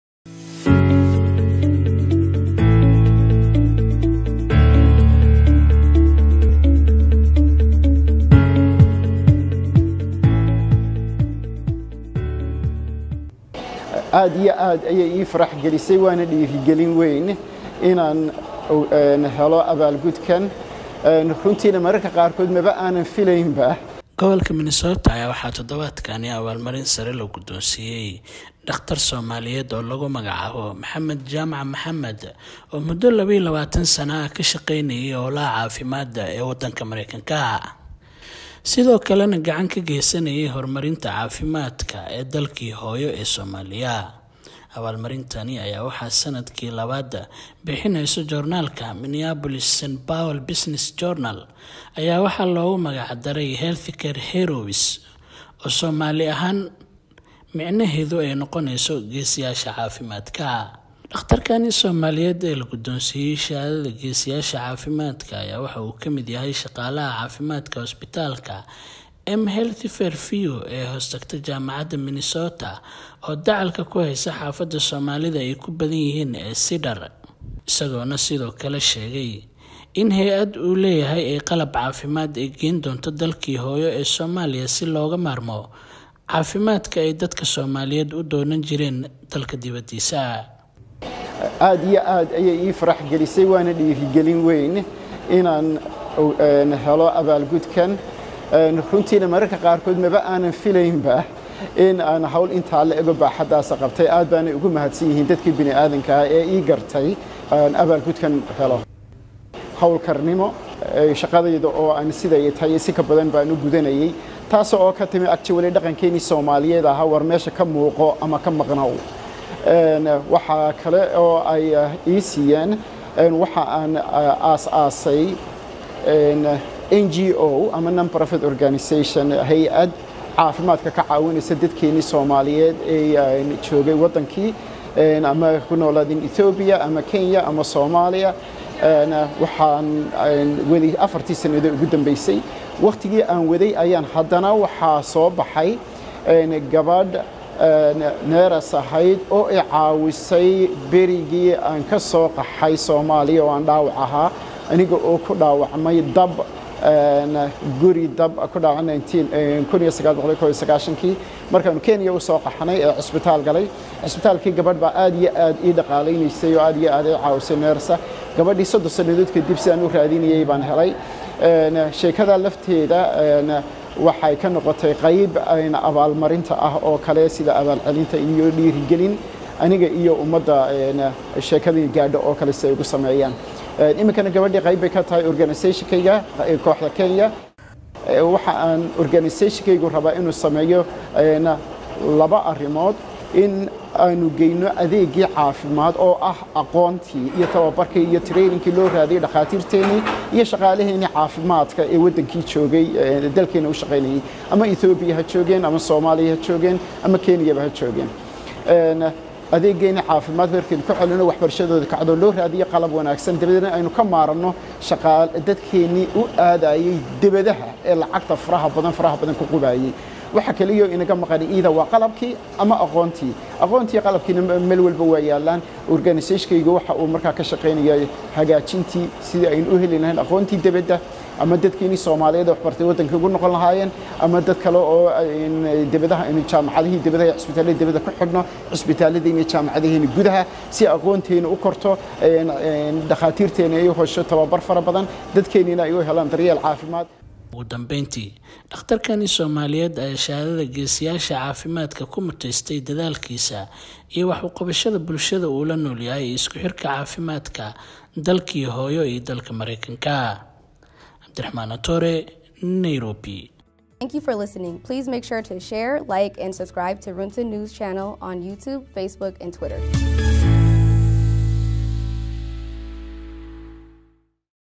2022 Healthcare Heros Award Winner
Dr. Mohamed Jama Mohamed of M Heath Fairview of Minnesota Receives this Year's Heros Health Care Award for his Outstanding Services. He is planning to deliver medical equipment to Somali so people will not need to travel abroad for medical emergencies.